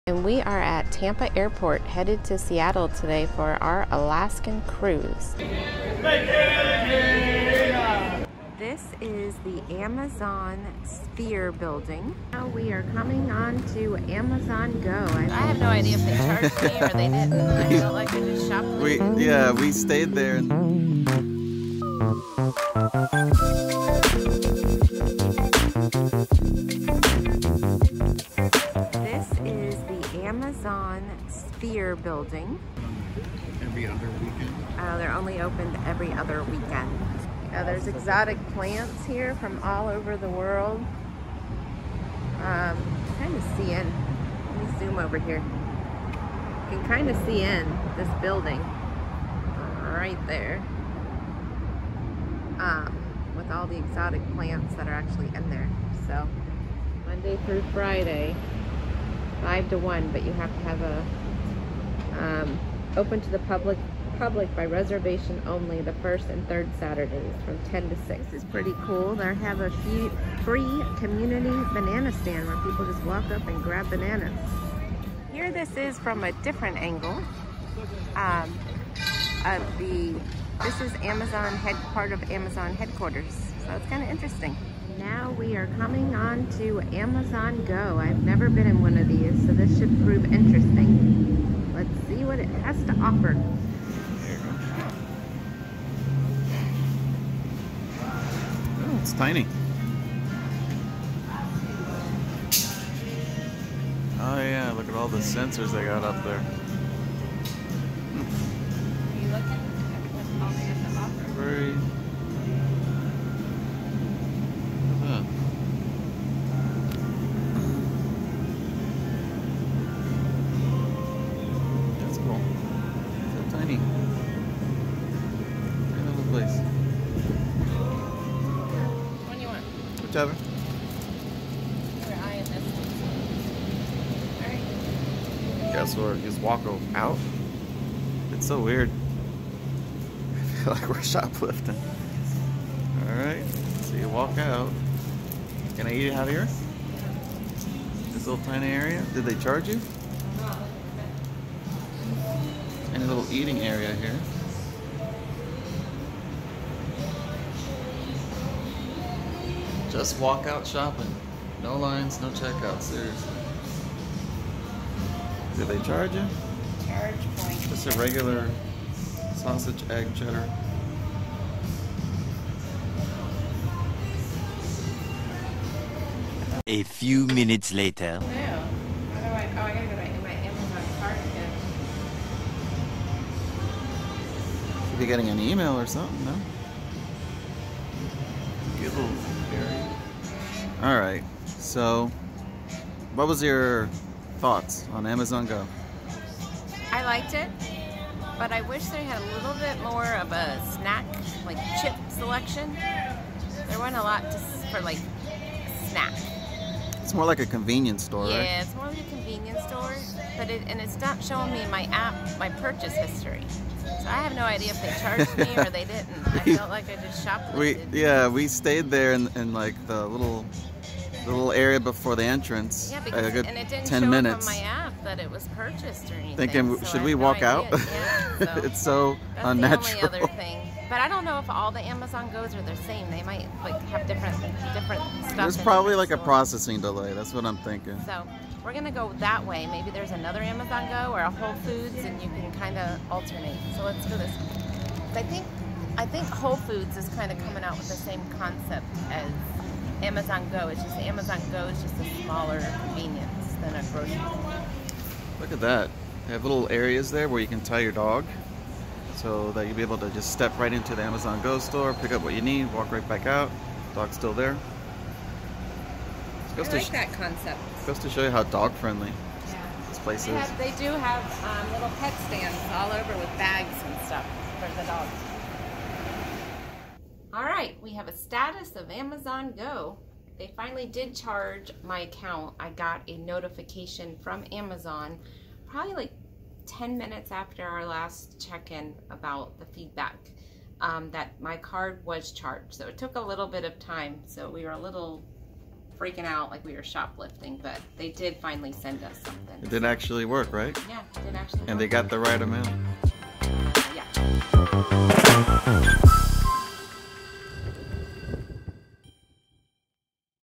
Seattle Spheres and Amazon Go
Day 2 of our Alaska trip deals with exploring more of Seattle's downtown area.  In particular, we check out the unique architecture of the Seattle Spheres and we try to not get arrested for shoplifting in the Amazon Go store.

For those that don't know, Amazon Go is a new shopping concept by Amazon that showcases advanced camera tracking technology to automatically charge a person with whatever products they carry out the store.